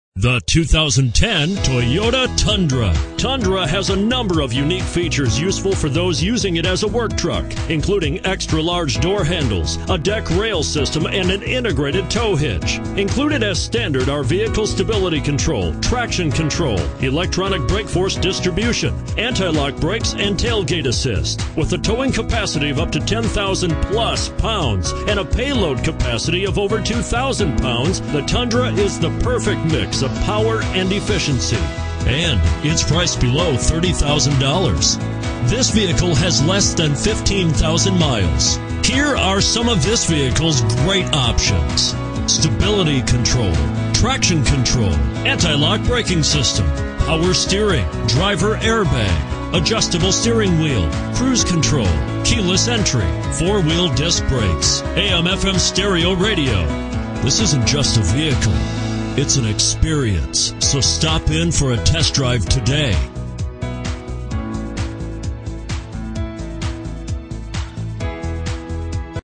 2010 TOYOTA TUNDRA Alvin, TX AX097154
2010 TOYOTA TUNDRA Alvin, TX
Stock# AX097154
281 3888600

Ron Carter Toyota (RonCarterToyota)
3507 Fm 528

***2010 Toyota Tundra CrewMax is ready to tackle any obstacle!*** MP3 Player, Stability Control, Multi-Zone A/C Call RON CARTER TOYOTA at . Visit roncartertoyota.com for full inventory.